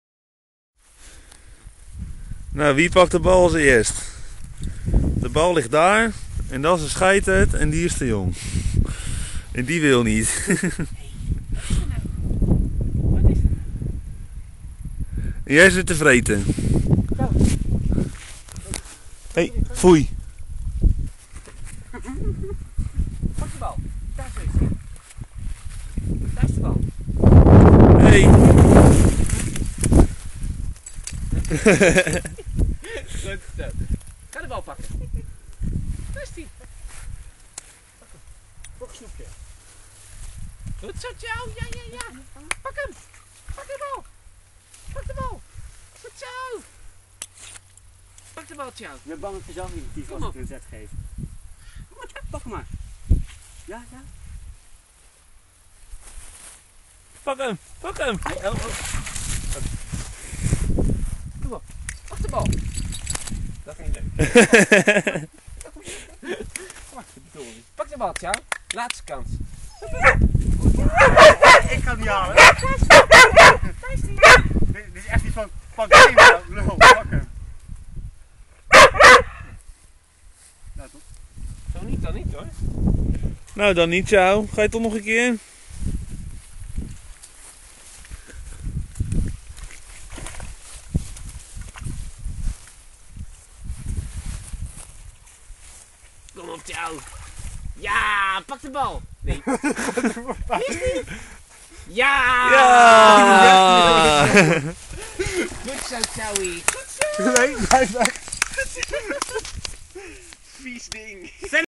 Senna en Cao proberen bal te pakken!
Waterhonden maar houden niet van water